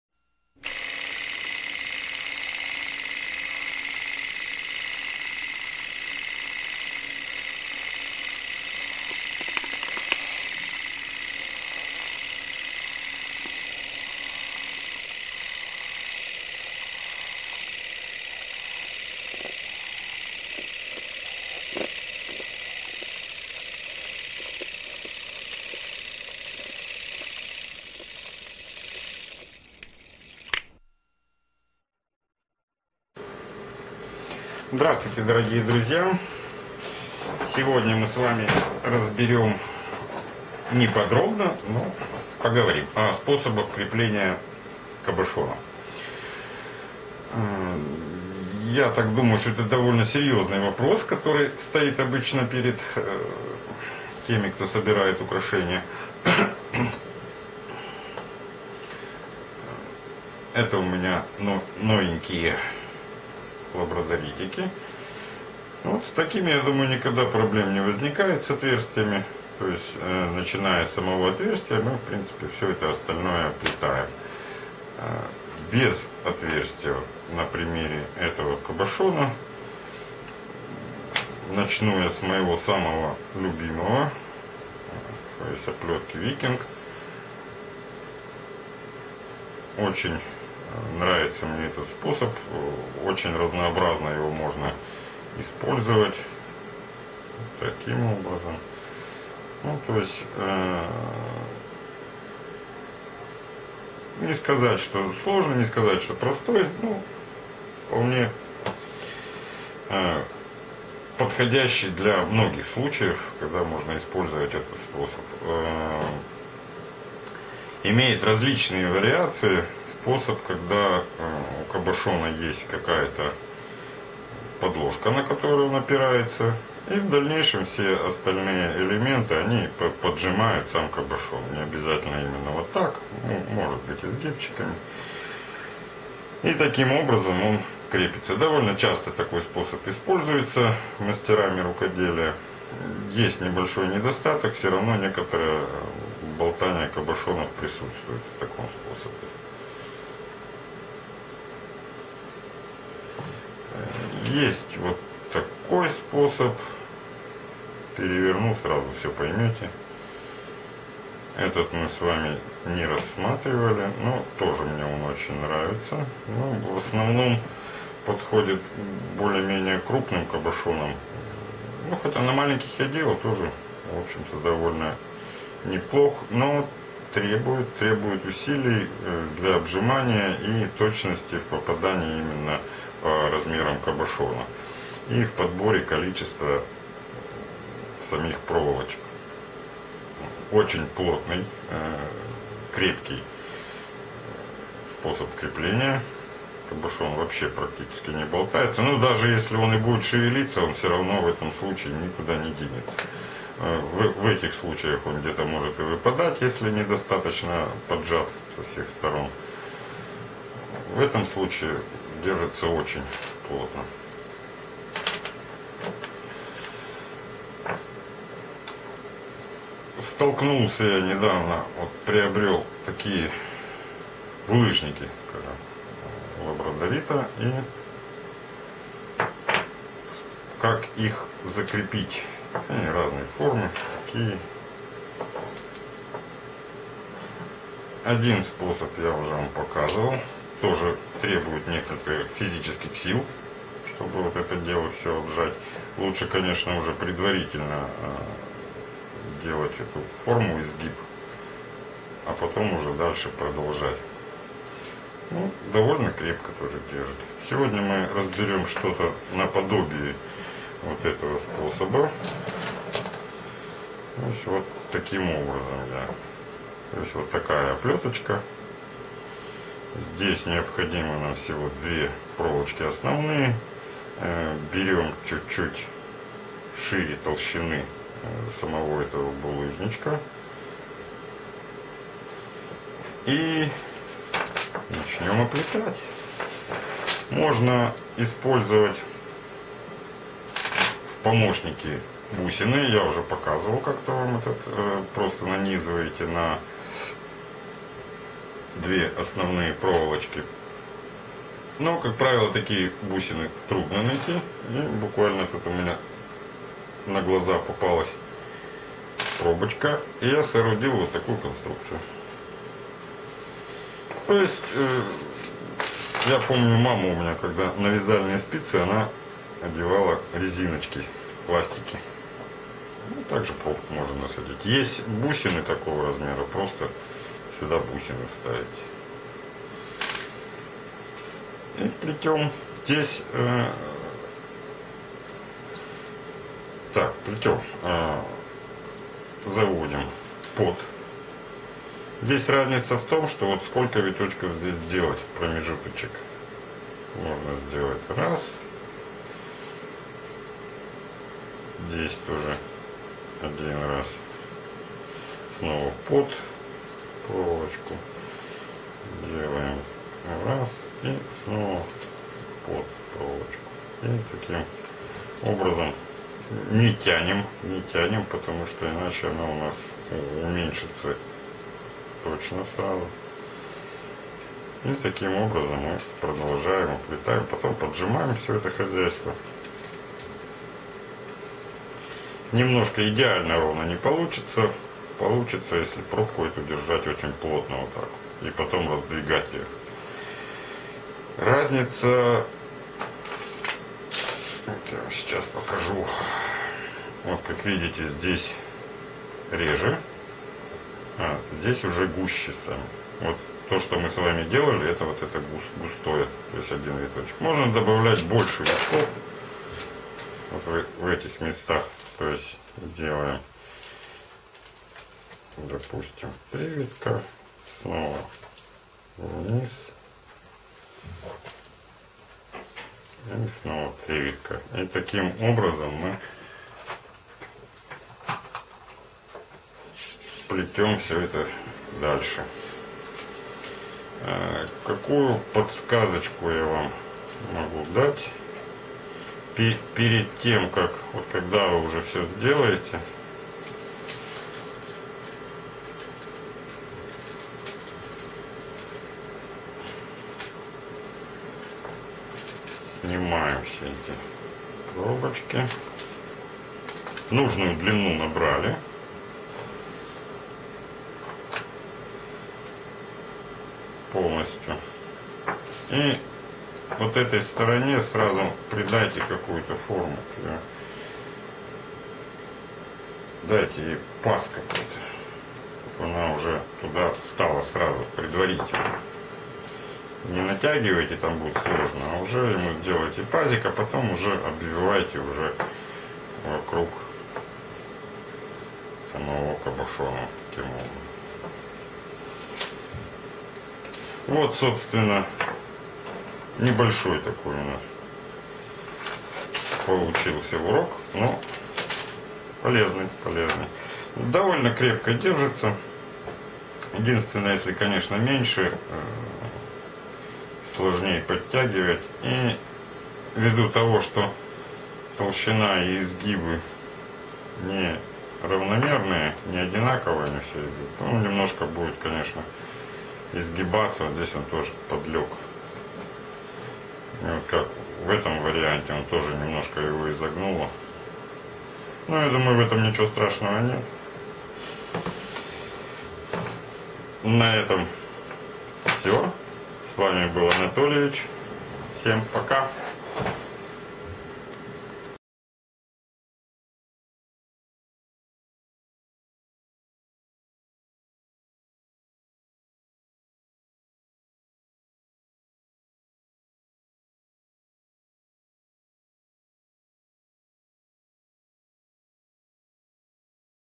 Крепление кабошонов Wire Wrapping
Крепление кабошонов Wire Wrap
Ожерелье  из проволоки в технике  Wire Wrapping.
Плетение подвески из медной проволоки своими руками. Плетение украшений из проволоки в технике Wire Wrap. Видео от Анатольевича для начинающих мастеров рукоделия.

Сайт «HandMade — заработок. Как заработать рукоделием»

«Рукодел» на Etsy